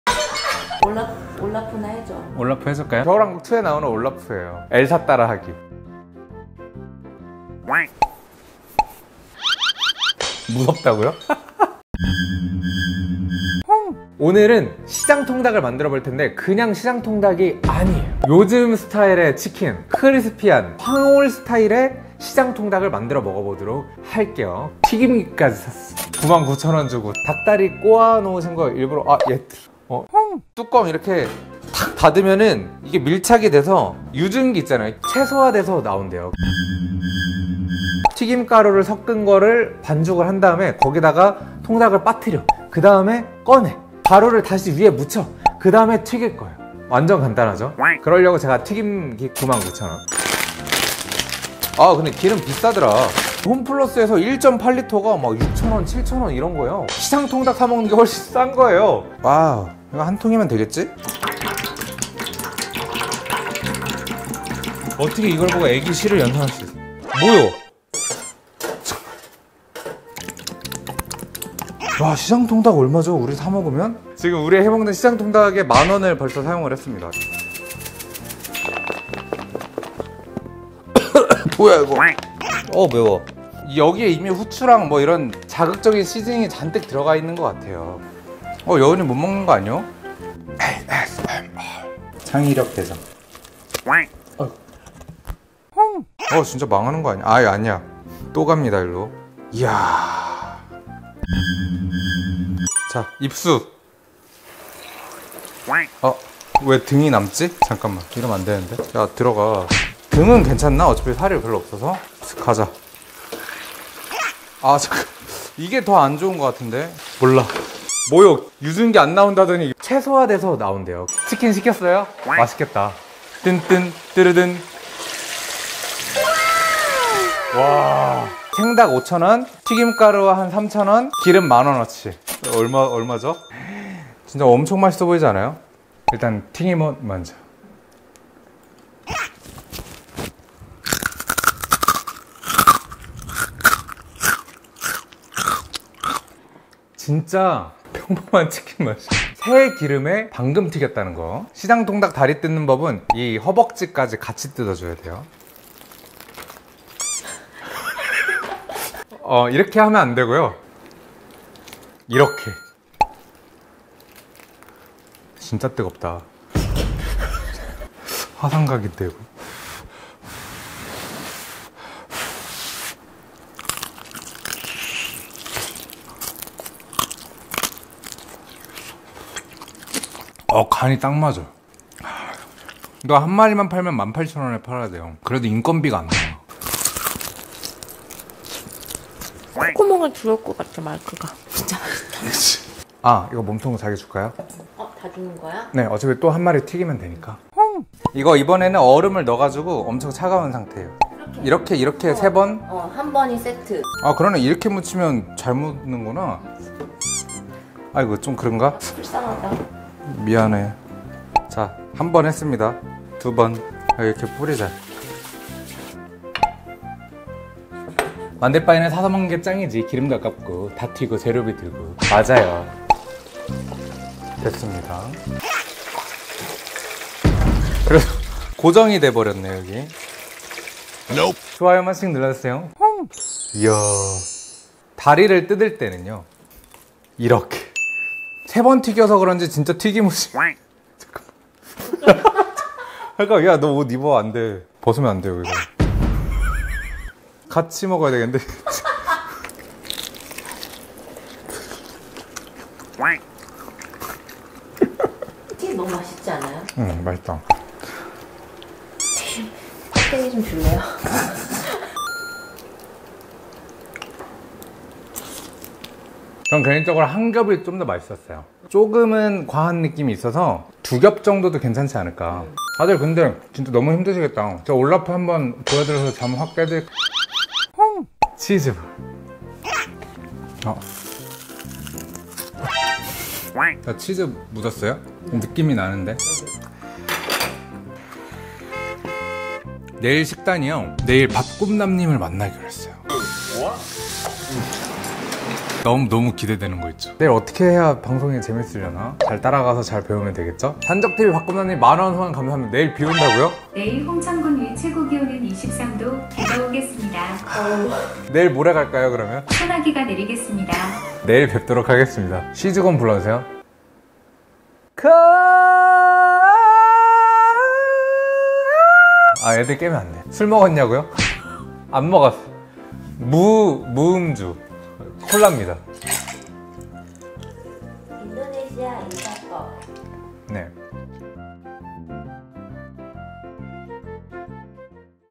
황금올리브 스타일 시장통닭 리얼사운드 먹방 | 올라프 홍라프 | 생방송 긍정부스터 | Old korean chicken EATING SHOW MUKBANG!
황금올리브 스타일의 시장통닭을 만들어먹어봤습니다!
치킨은 사먹는게 최고입니다 ㅋㅋㅋ ㅠㅠ